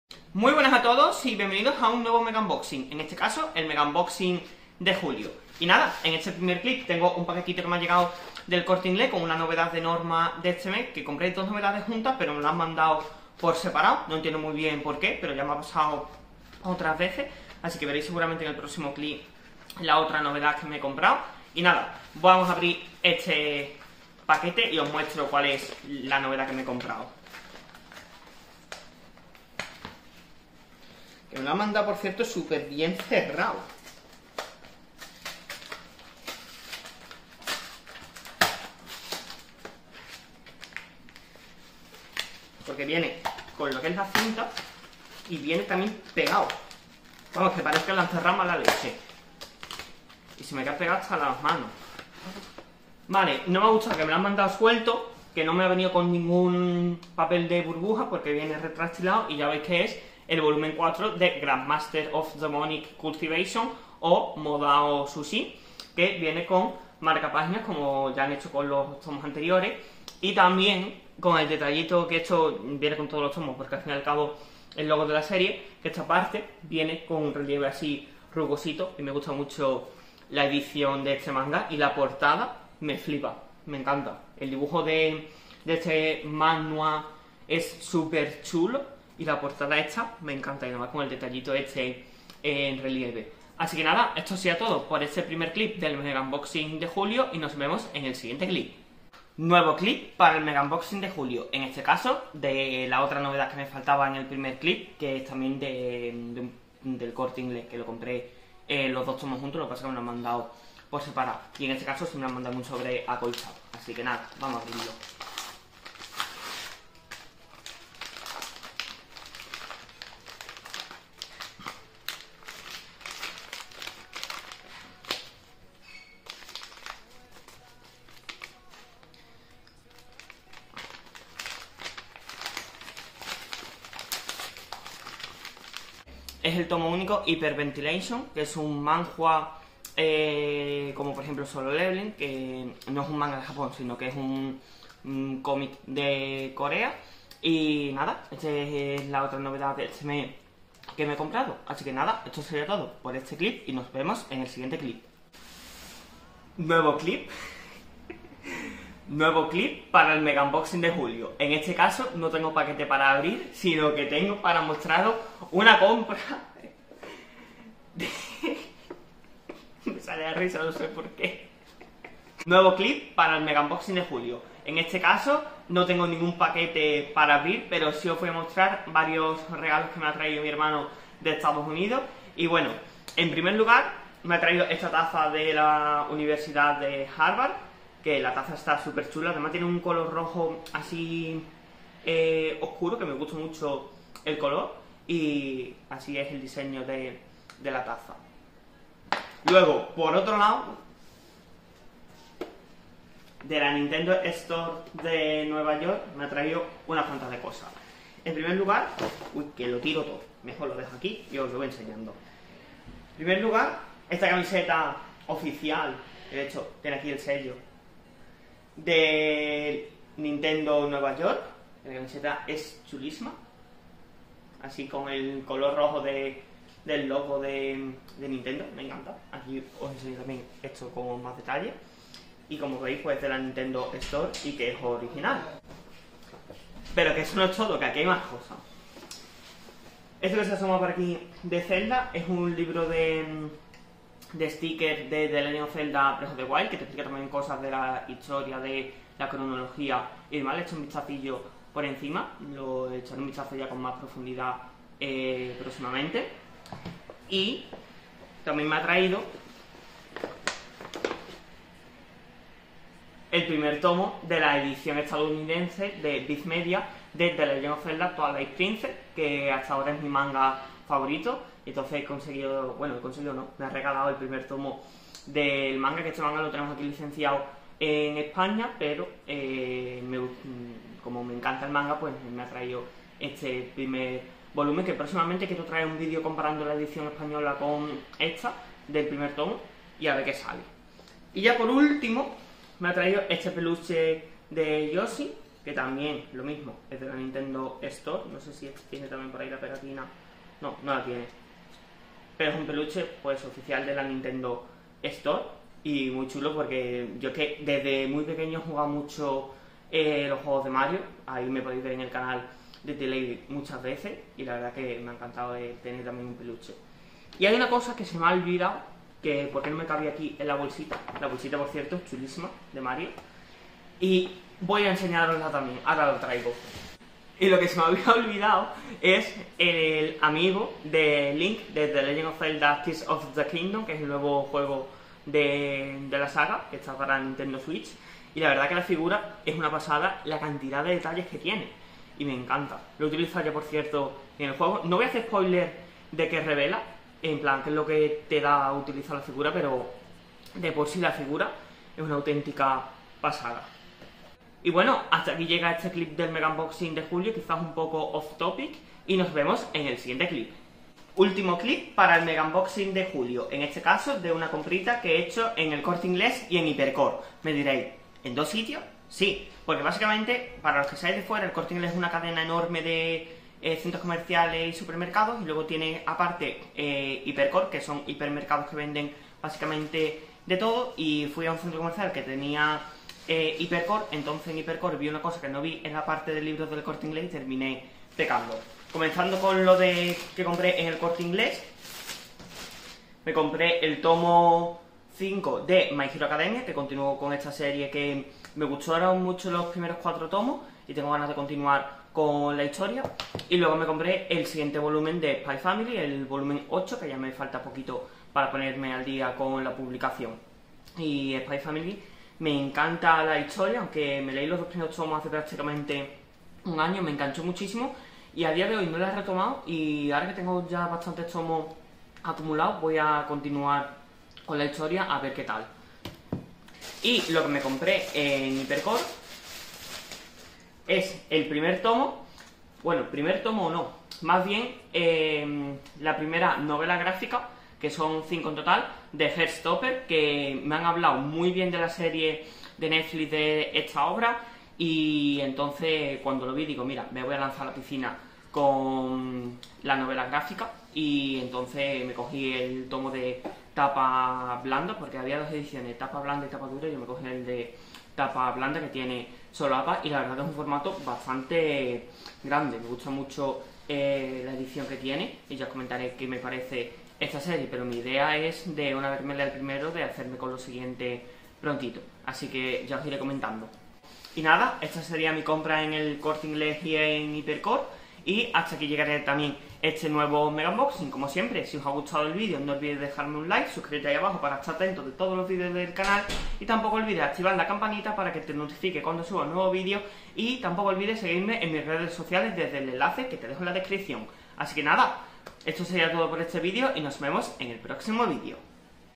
Mega Unboxing Julio 2023
Hola!
Aquí os traigo un nuevo Mega Unboxing cargado de manga y con un clip bastante especial. Para los que lleguéis nuevos, los Mega Unboxing son vídeos que hago cada mes donde recopilo todos los mangas que me van llegando y a veces alguna cosilla extra (como en este caso).